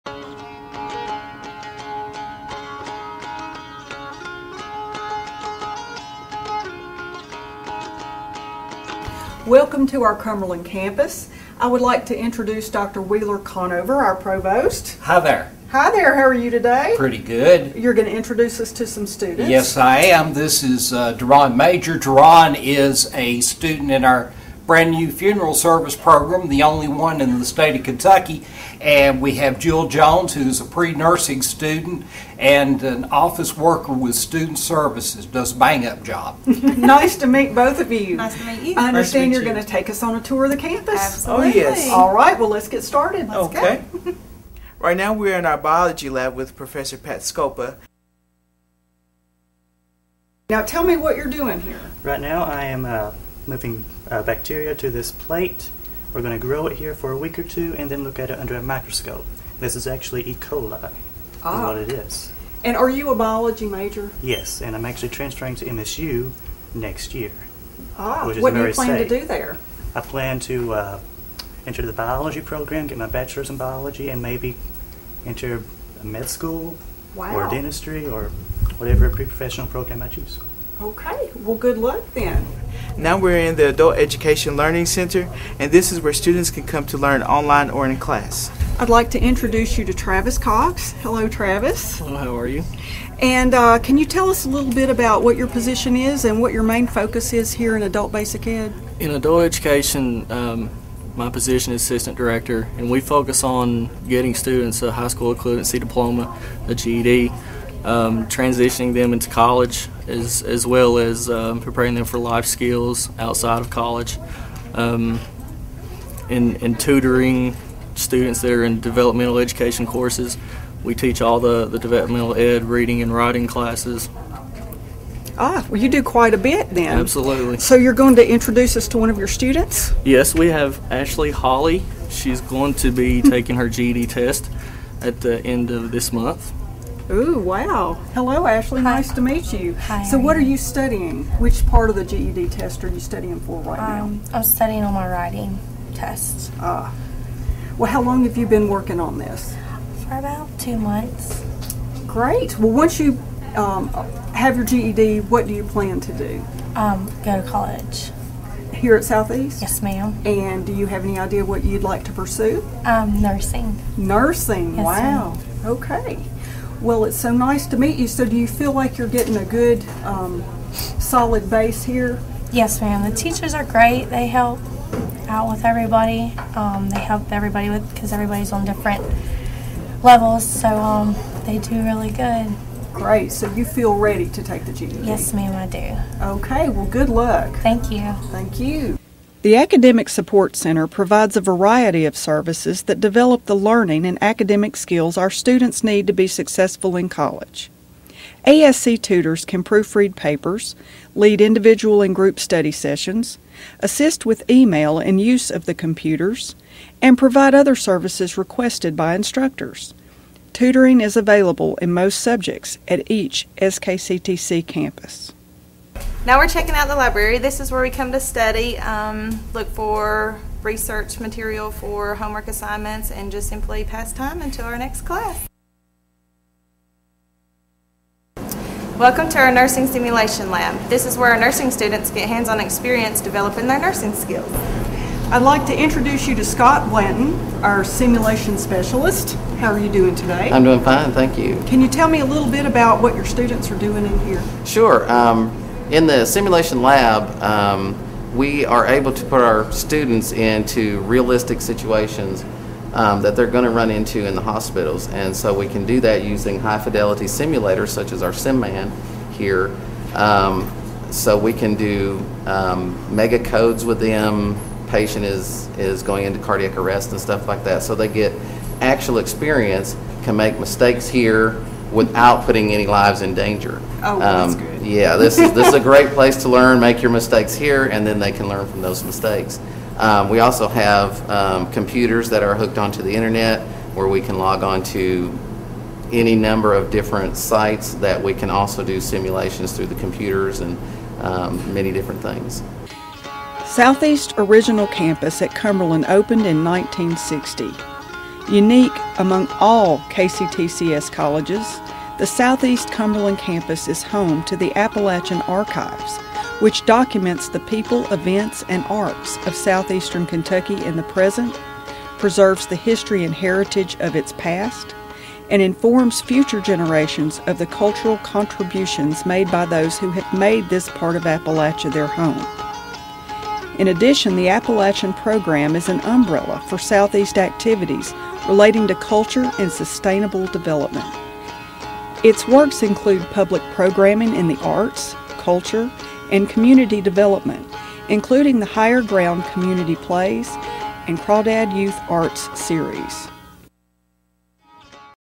Southeast Kentucky Community & Technical College Cumberland Campus
Welcome to Southeast Kentucky Community & Technical College.  Join our student ambassadors as they lead you on a guided tour of the Cumberland campus.